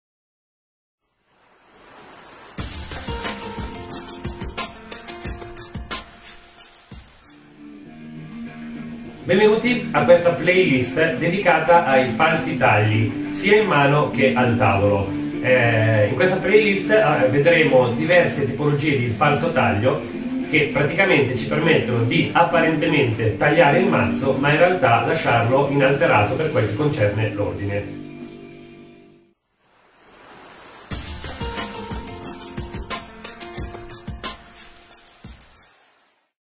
Introduzione ai falsi tagli
Diego presenta una playlist molto importante e molto richiesta, quella sui falsi tagli.